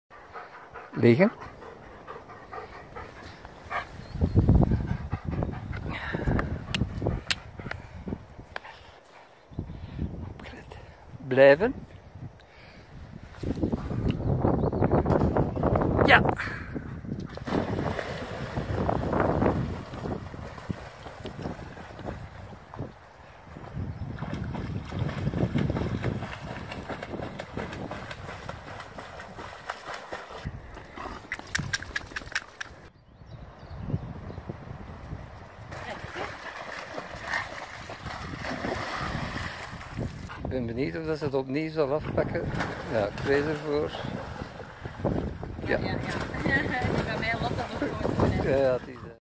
20190407 Chaam
Wandeling in het losloopgebied de Chaamse bossen (NL)